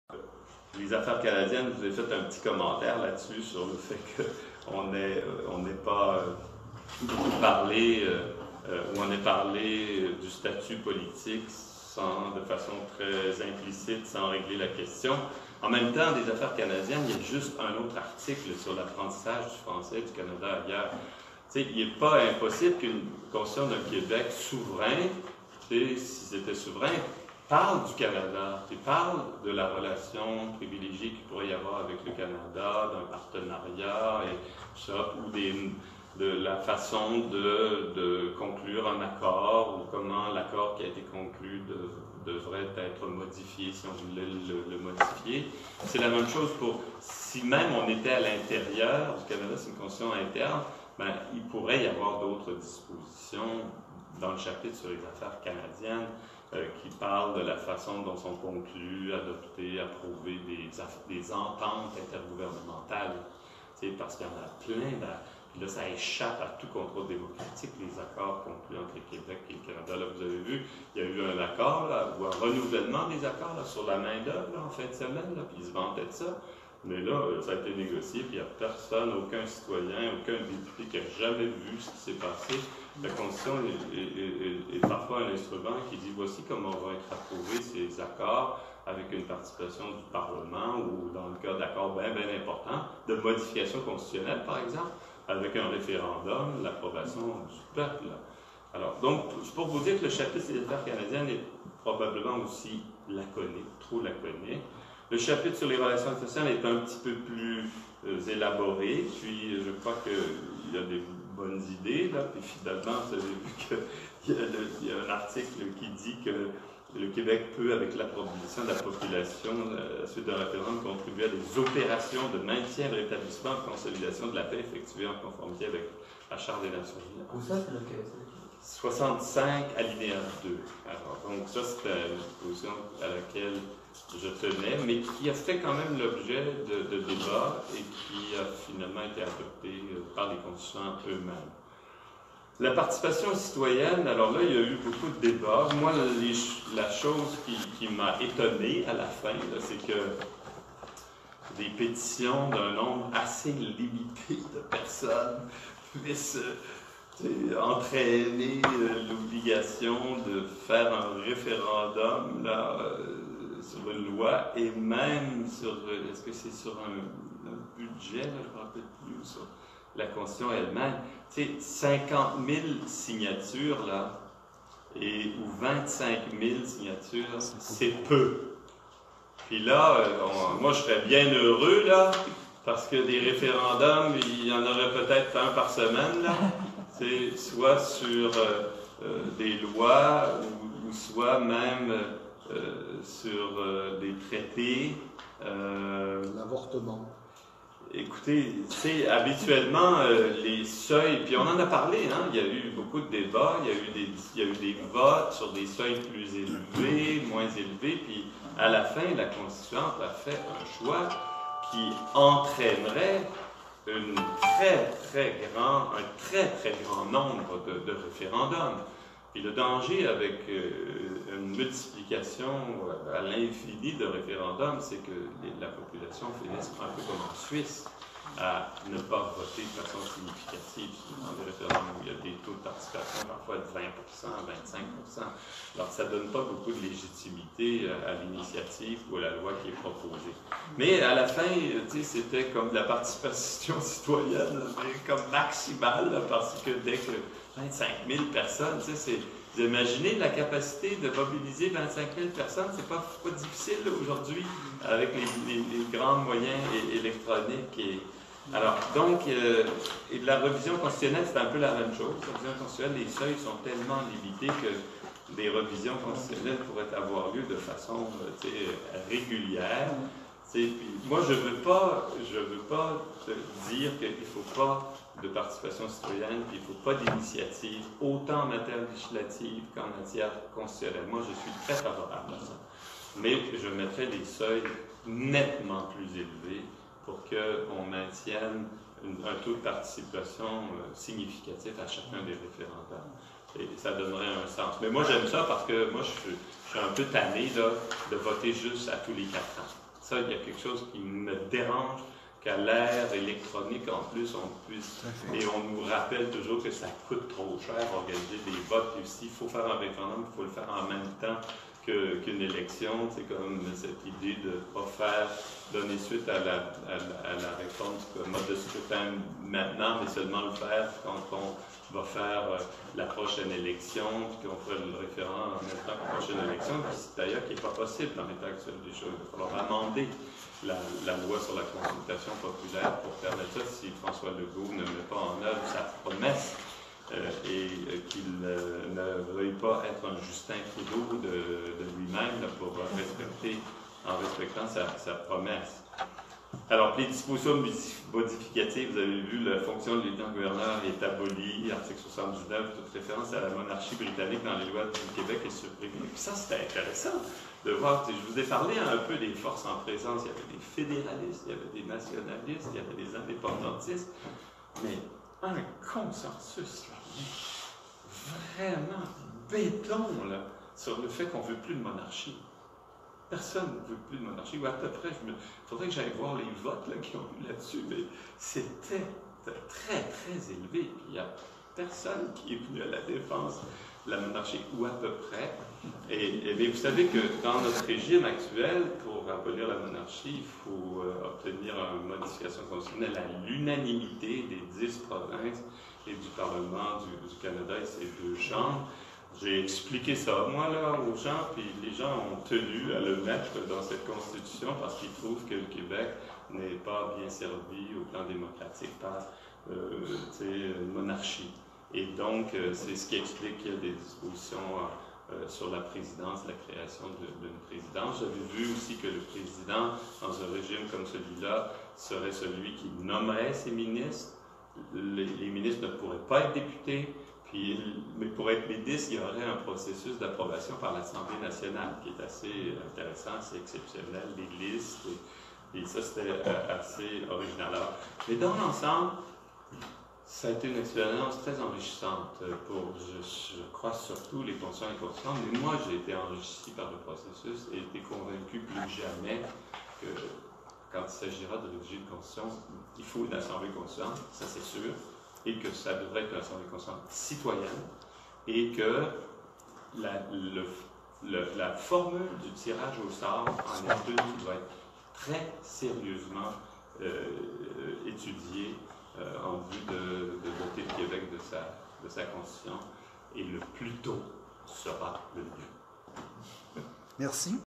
#08. Art 62 79 Aff Canada Rel Intl Particip Citoyen
Extrait #08 de l'allocution de Daniel Turp à l'assemblée constituante du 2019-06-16, au centre Afrika.